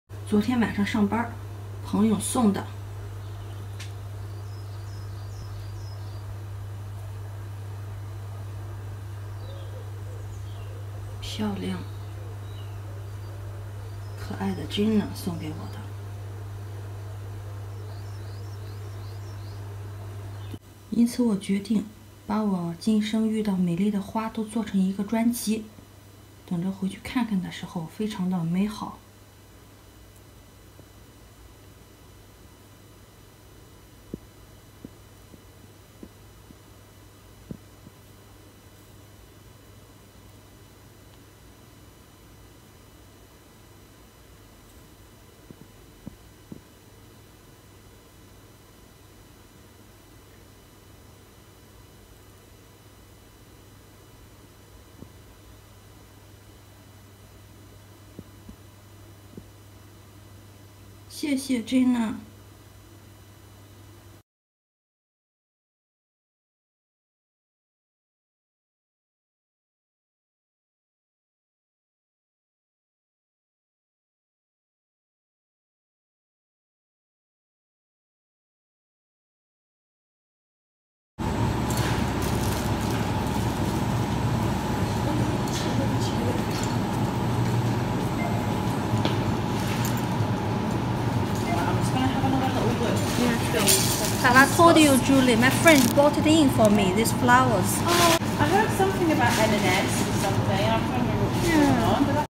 就在昨天晚快要下班的时候突然#意外收到惊喜😊so happy ！
Thank you so much lovely ! Lovely 珍娜！
这些天上班随处可以感觉到到处都是可爱的笑脸。
快要下班的时候没想到朋友突然出现（说是朋友其实就是见面打招呼、互相喜欢却没有电话号码也没有出去喝茶的朋友）送给我鲜花。

也许等到我退休的那一天回想起这美丽的时刻也会感到非常的暖心……